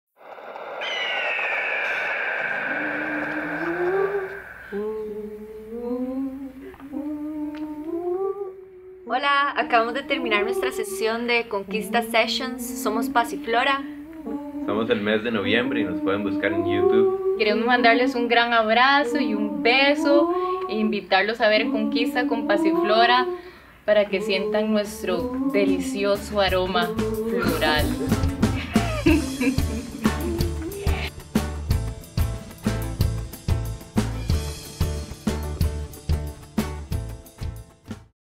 Conquista Sessions - Passiflora Invita
Passiflora te invita a disfrutar de su aroma floral y esencia gitana en el próximo Conquista Sessions.  El episodio 010 es dirigido por Gustavo Cosenza.

Estreno: 12 de Noviembre del 2013.